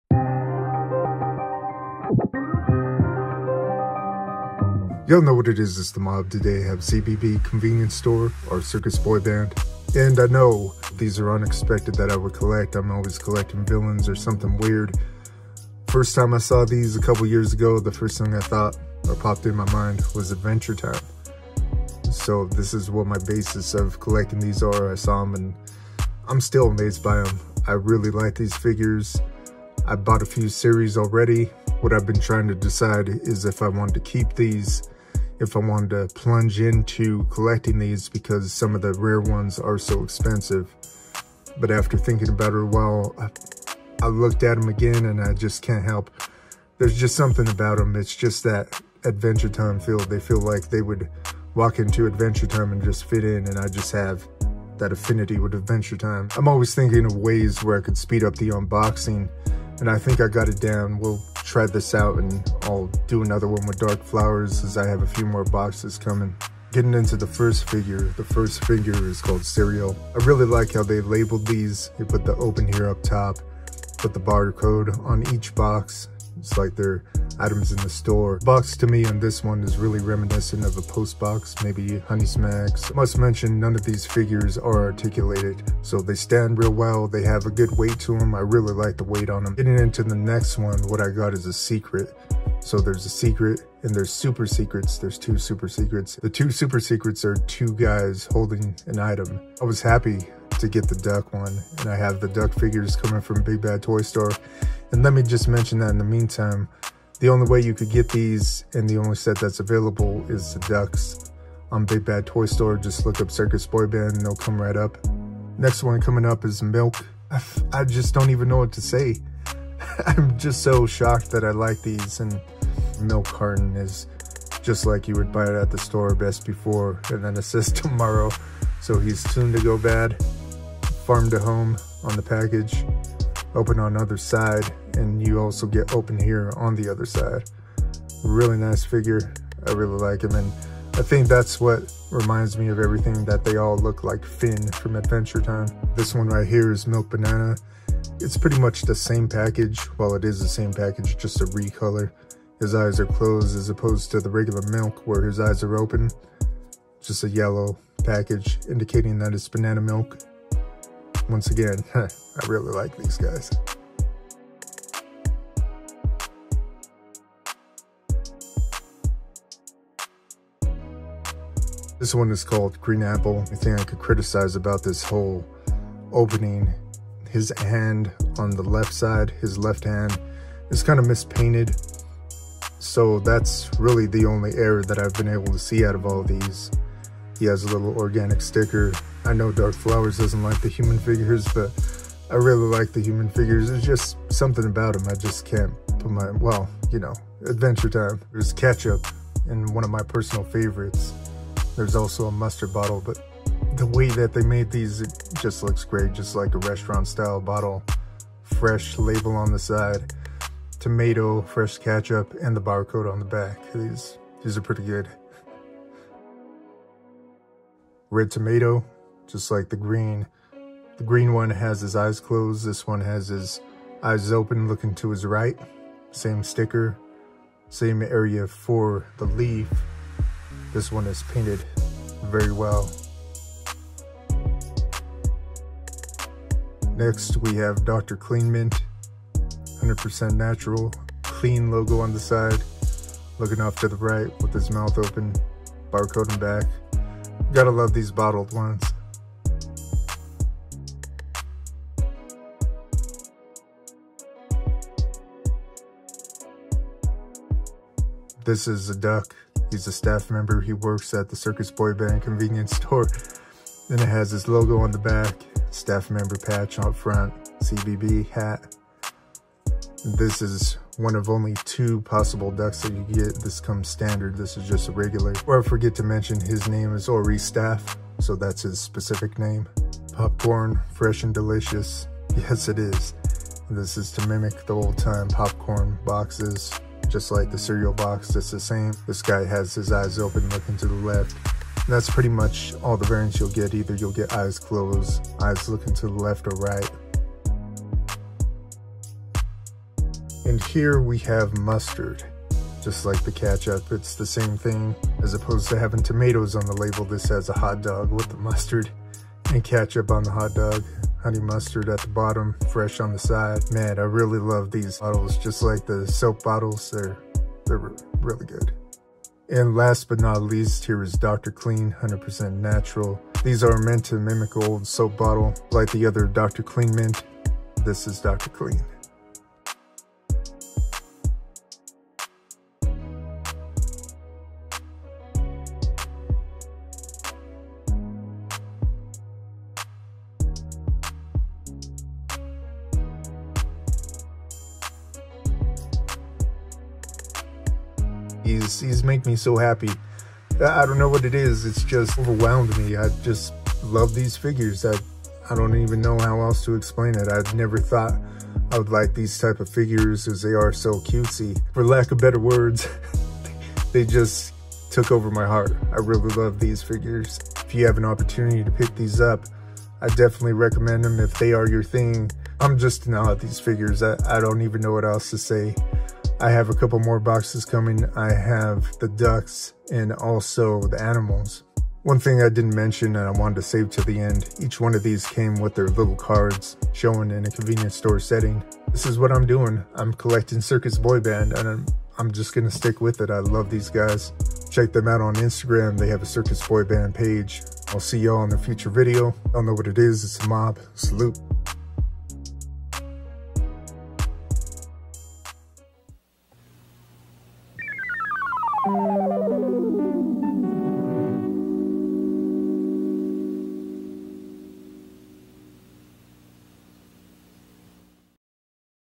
CBB Convenience Store Case
goblinmob opening of a case of circus boy band convenience store figures….dont know why this has been copyrighted twice.